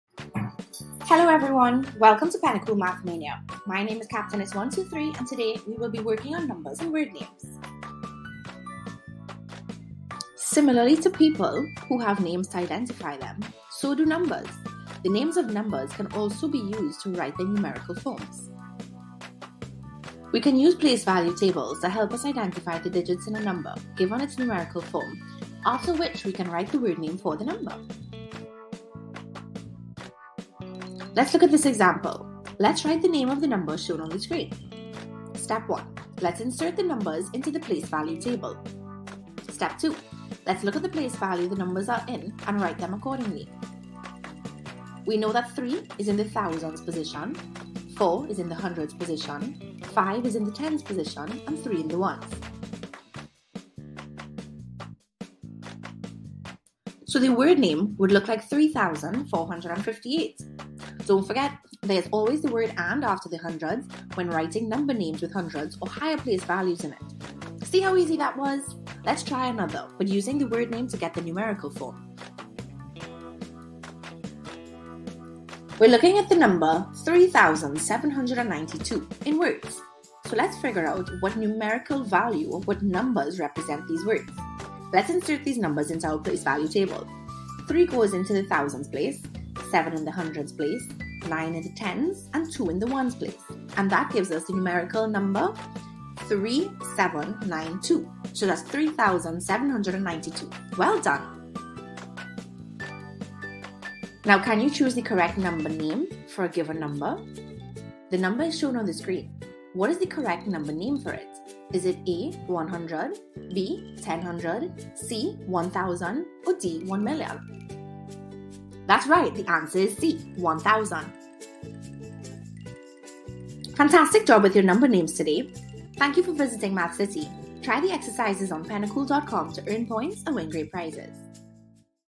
Numbers and Their Word Names (Standard 3) - pennacool.com
Join Captainess 123 as she tells us about numbers and their words names.

Numbers - Word Names - Standard 3

Log on to pennacool.com 123 Math & Language Arts Platform to access:
- FREE
- Self-correcting
- Curriculum aligned exercises
- Immediate Feedback